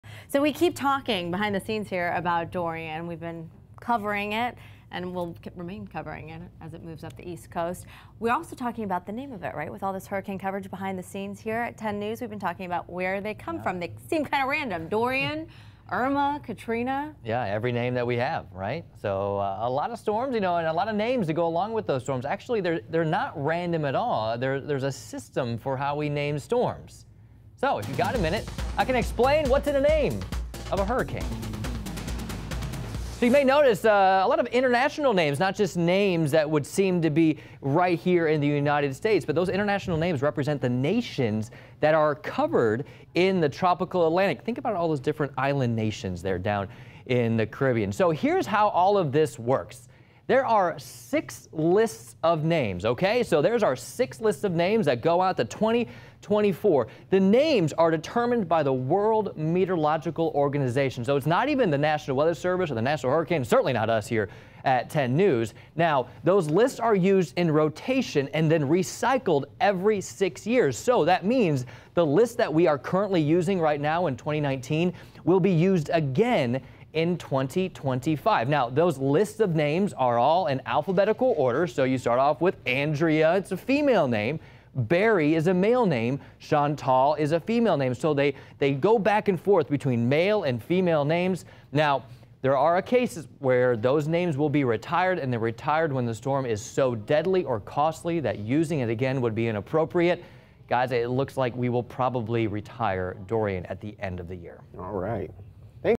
Got a Minute: How hurricanes are named
10News meteorologist Grant Gilmore takes a look at how hurricanes are named and why some storm names are retired and others are not.